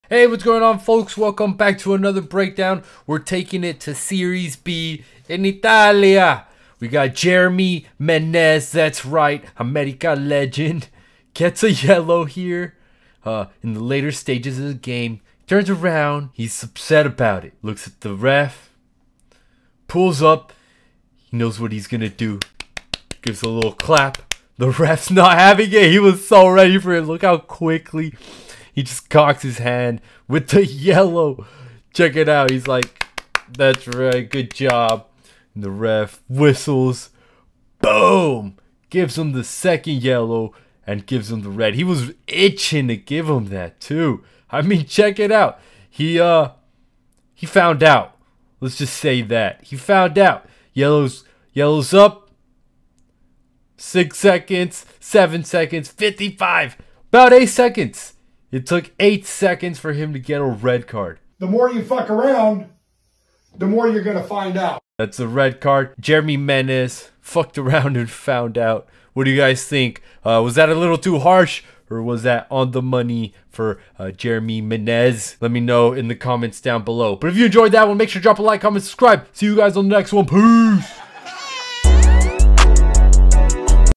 French player gets two yellow cards in 6 seconds for clapping, a breakdown.
🔴⚽ Prepare to witness one of the most astonishing moments in football history as French striker Jeremy Menez finds himself on the wrong end of a referee's decision. In a match that left fans and players alike in disbelief, Menez managed to receive not one, but two yellow cards in just six seconds! 😱

👏 What led to this unprecedented turn of events? It all started with Menez's sarcastic clapping, a gesture that did not sit well with the match official. In a surprising twist, the referee responded by brandishing the yellow card not once, but twice, leading to Menez's unexpected early exit from the game.

⚡ Join us as we break down the jaw-dropping sequence of events, analyze Menez's actions, and discuss the aftermath of this bizarre incident. Football enthusiasts, this is one video you don't want to miss! Hit the play button now and unravel the drama that unfolded on that unforgettable Tuesday.